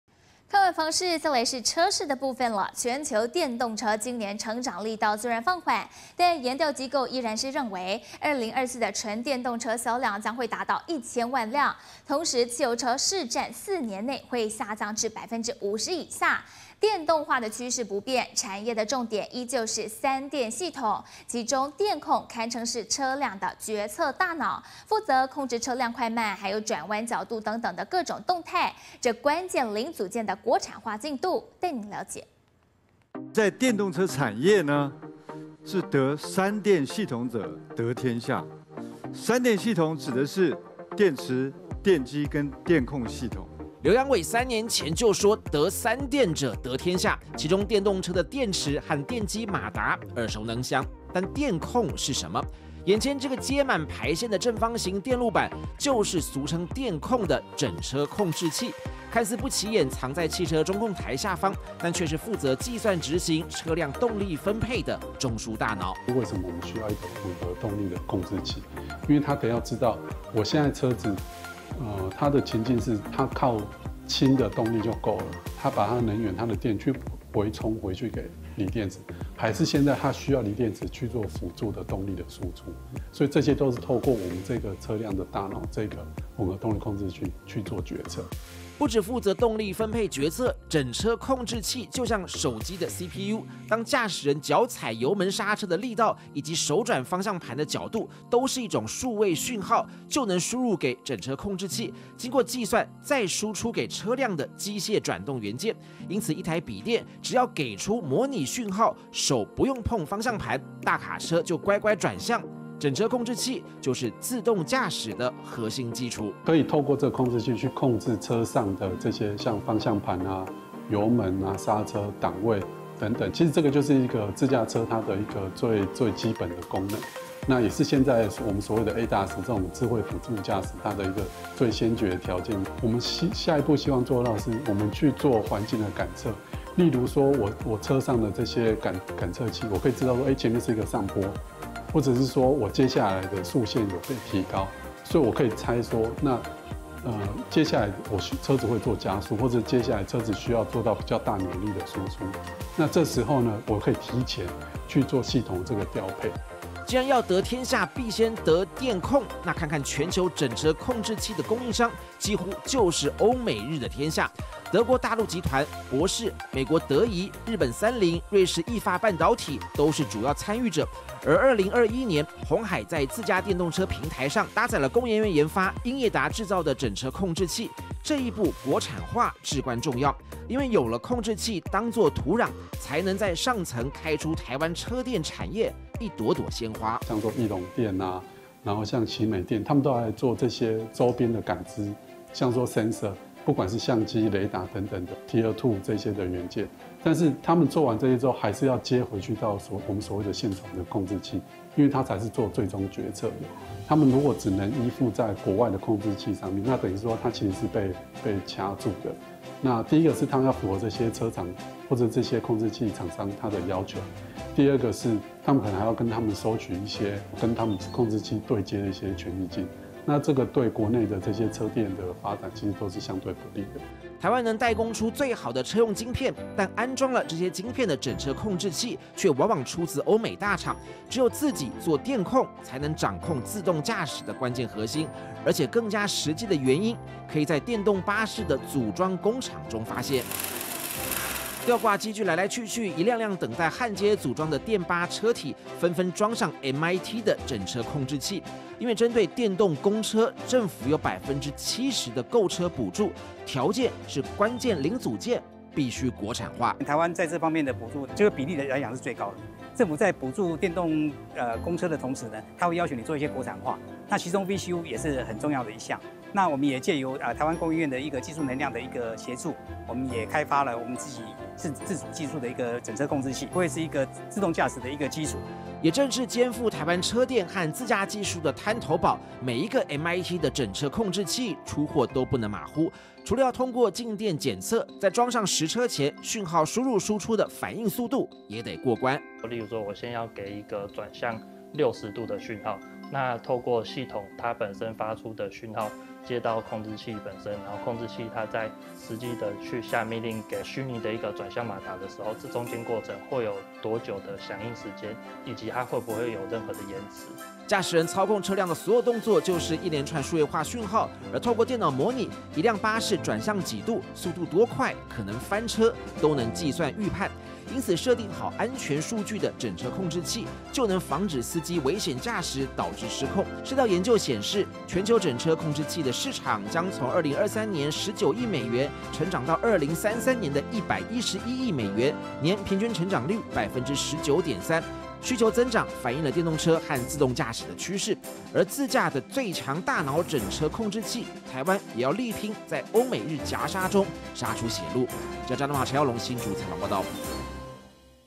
得三電系統者得天下！國產「電控」力抗歐美日
全球電動車今年成長力道雖然放緩，但研調機構依然認為，2024純電動車銷量將達到 1000萬輛，同時汽油車市佔四年內會降至50%以下。電動化趨勢不變，產業重點依舊是「三電系統」，其中「電控」，堪稱是車輛的決策大腦，負責控制車輛快慢以及轉彎角度等各種動態，這關鍵零組件的國產化進度如何，帶您了解。